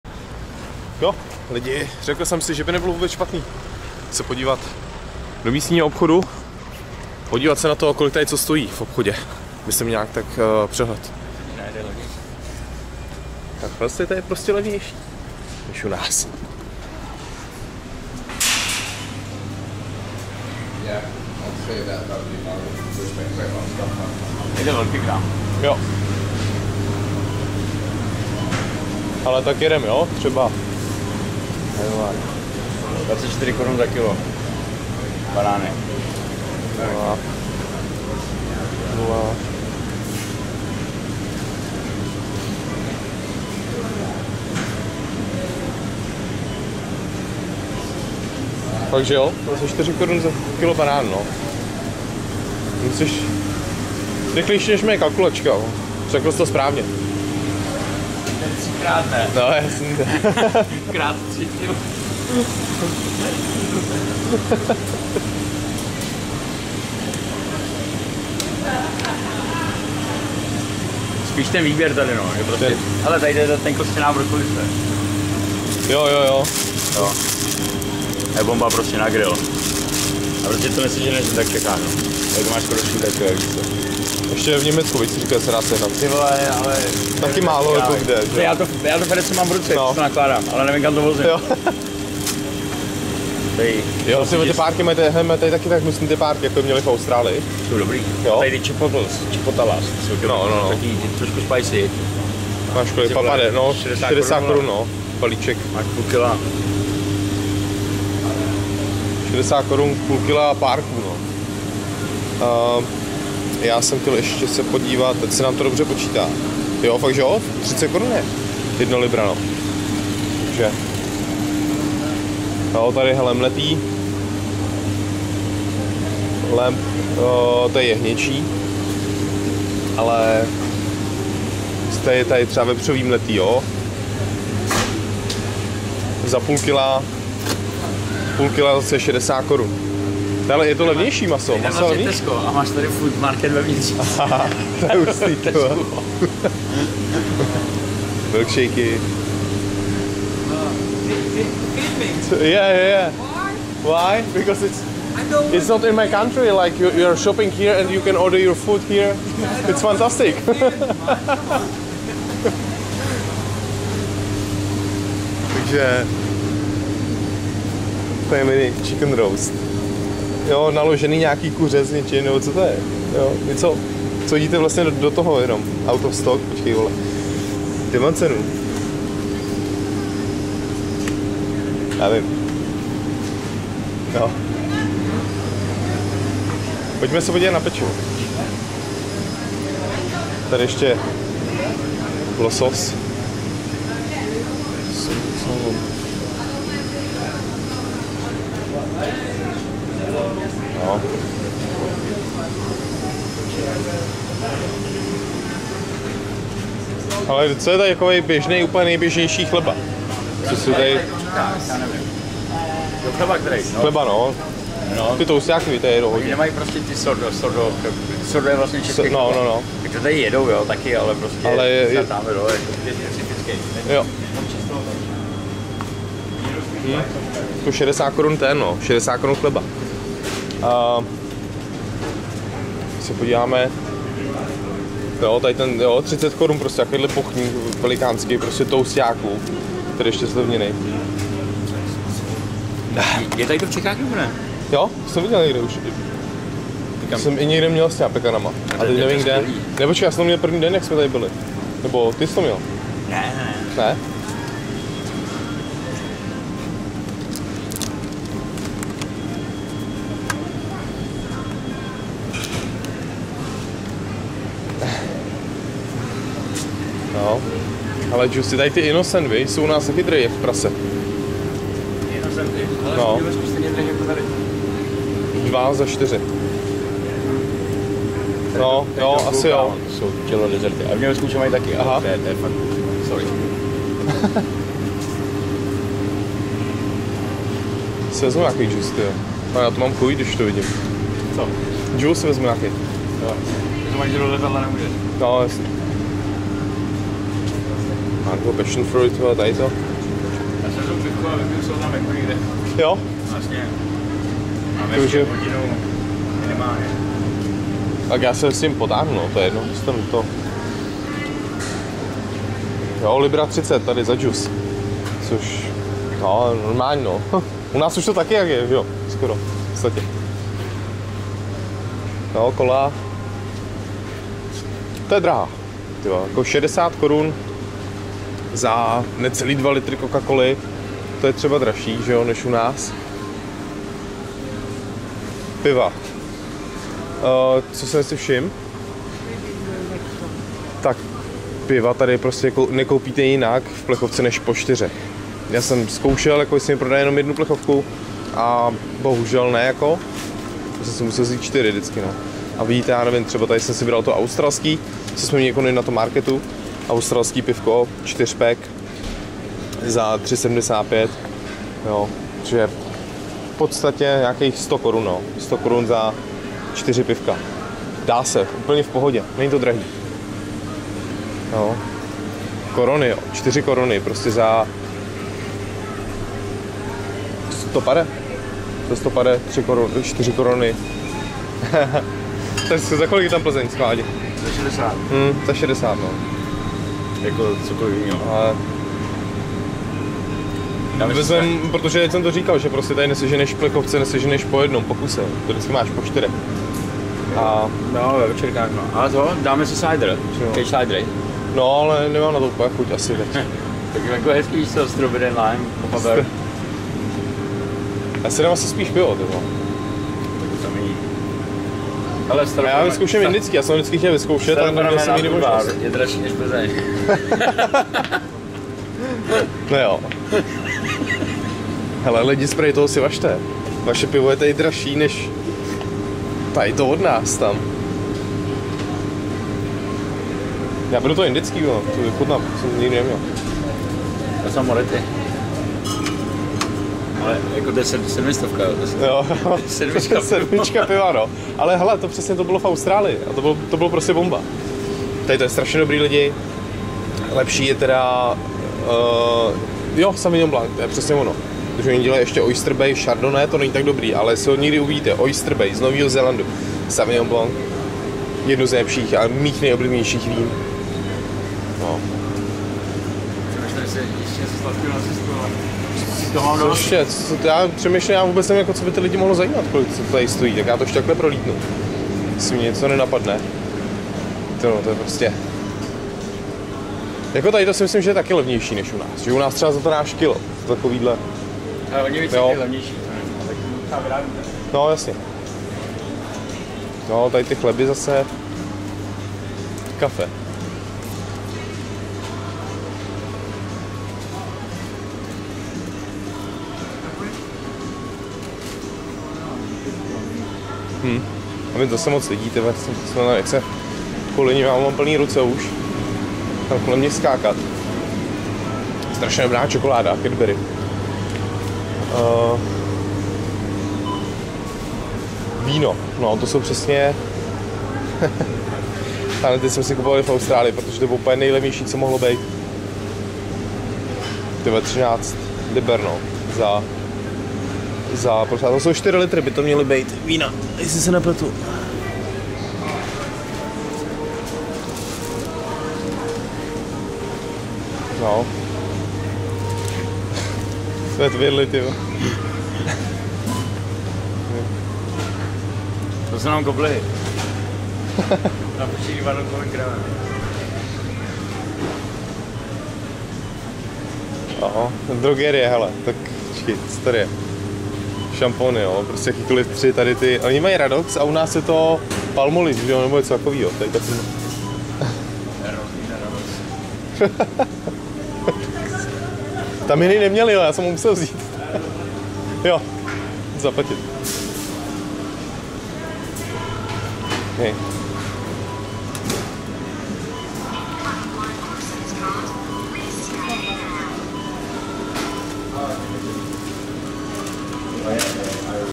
Je v Anglii levněji než v ČR? Podívali jsme se na ceny potravin 🇬🇧
Zavítali jsme v Londýně do obchodního řetězce Morrisons, abychom zjistili ceny potravin v Anglii. Možná budete překvapeni :-)